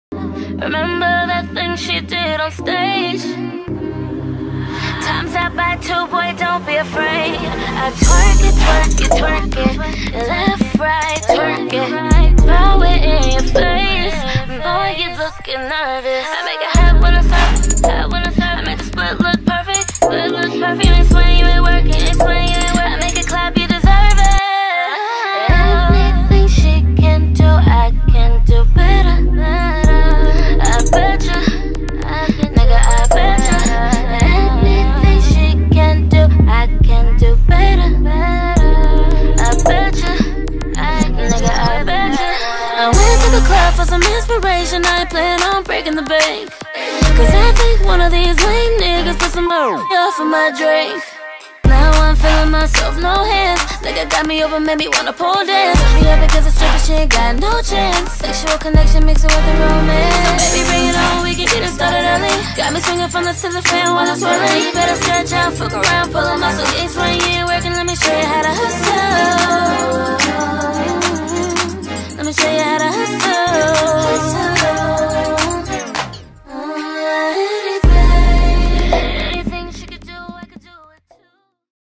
(Cisco Choreography) @ PlayGround LA
This is class footage from when I took Cisco heels class. Yes “Heels Class” , I get to challenge myself in making the choreography a masculine piece rather then a feminine one. It tests not only the ability to master my masculinity but my creativity as well. Having to literally create on the spot while he’s teaching the women the feminine part of it, I have to make it my own and recreate the movement to fit my body. Hope you guys enjoy.